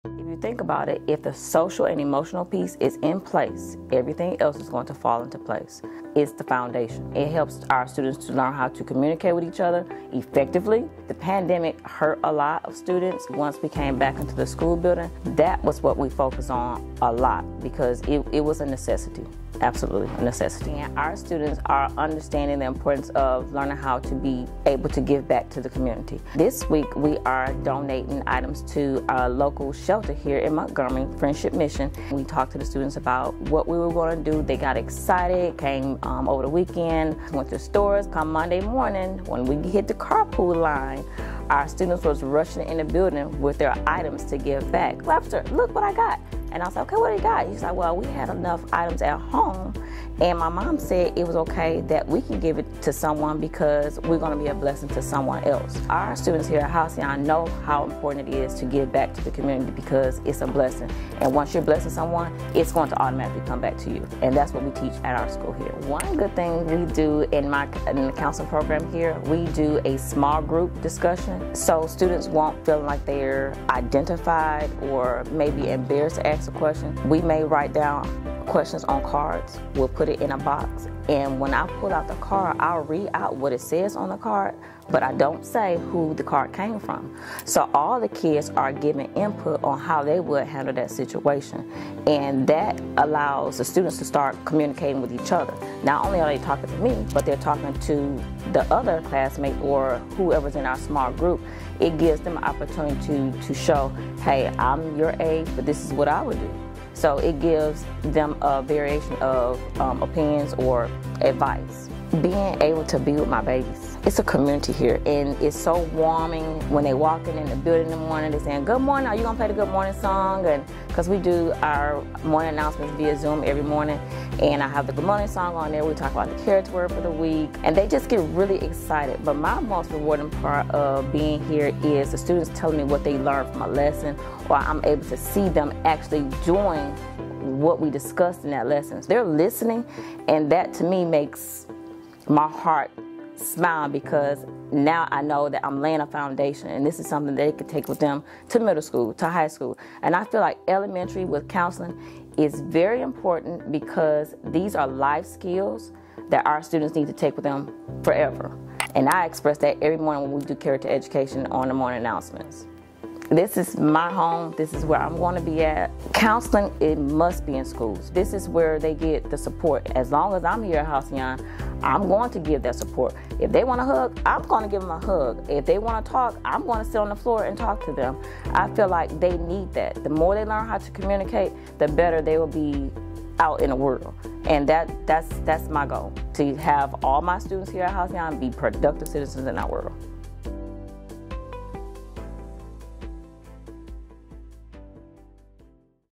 MPS Voices: Halcyon Elementary Counselor Shavon Webster
"Our students here at Halcyon know how important it is to give back to the community because it's a blessing. And once you're blessing someone, it's going to automatically come back to you."

In this MPS Voices video,  Halcyon Elementary counselor Shavon Webster talks about creating a social and emotional foundation that students can build upon for the rest of their lives.